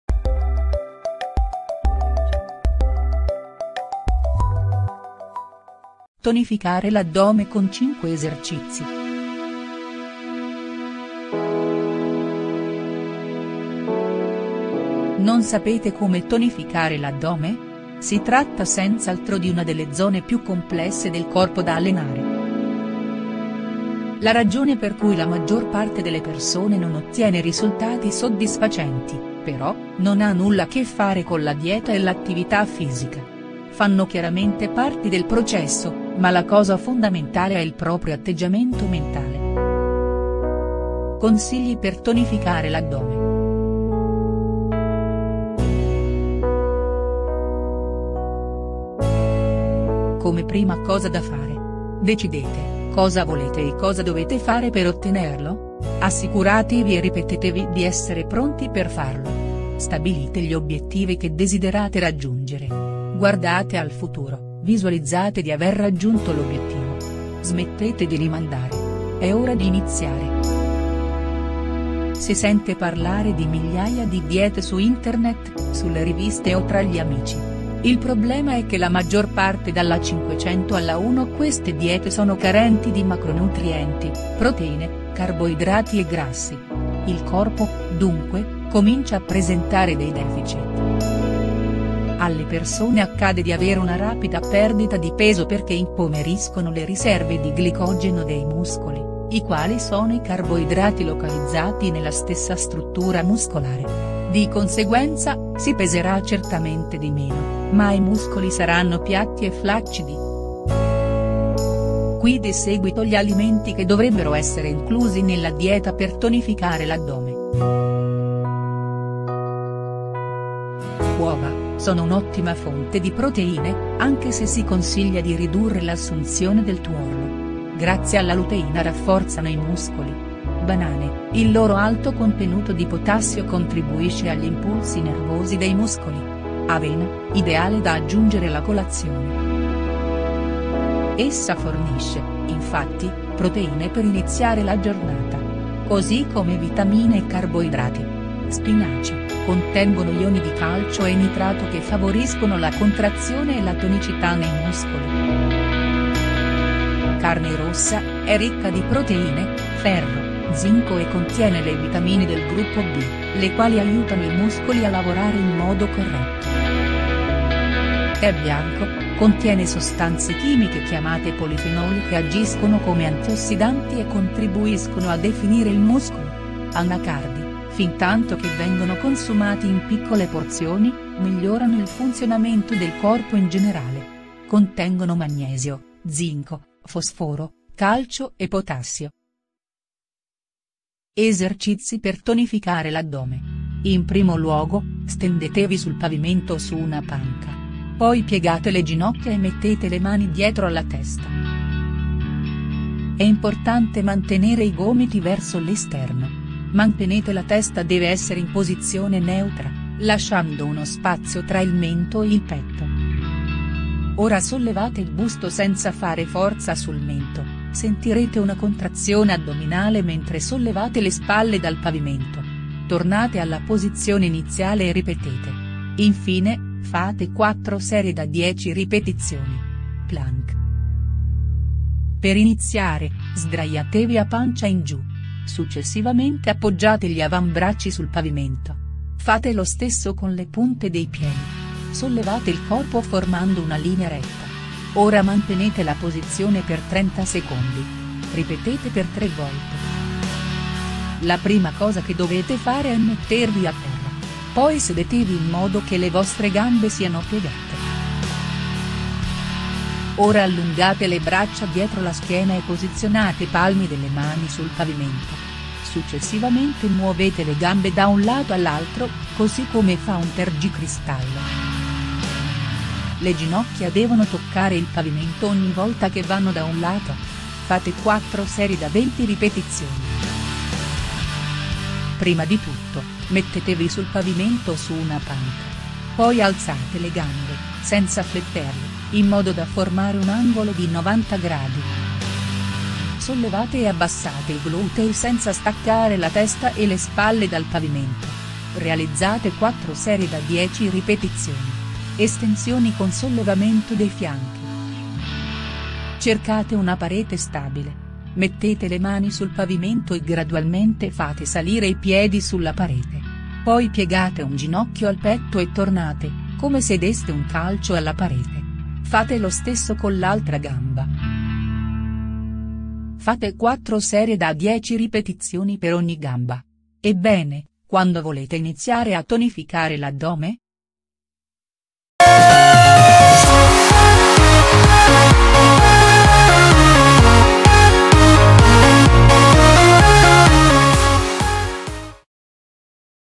Tonificare l’addome con 5 esercizi - Salute 365
Un addome tonico non equivale a passare ore in palestra o a fuggire dai carboidrati. Il segreto consiste nel combinare proteine, grassi sani e attività fisica.